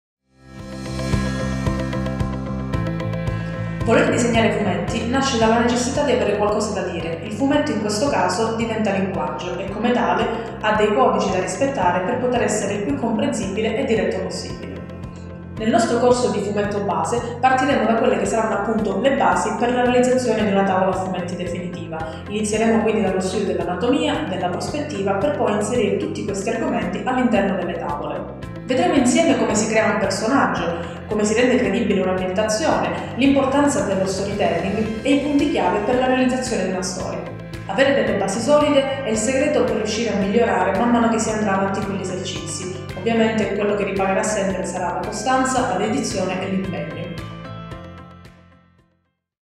Corso di Fumetto Online in italiano
Il corso prevede l’apprendimento e l’esecuzione delle conoscenze base per la realizzazione di una tavola a fumetti ed è rivolto a tutti gli appassionati della nona arte, indipendentemente dal tipo di studi conseguiti, età, ed esperienza.

Il corso, unico nel suo genere, è completamente in italiano è composto da videolezioni consultabili da qualsiasi dispositivo tutte le volte che si desidera e da contenuti esclusivi scaricabili.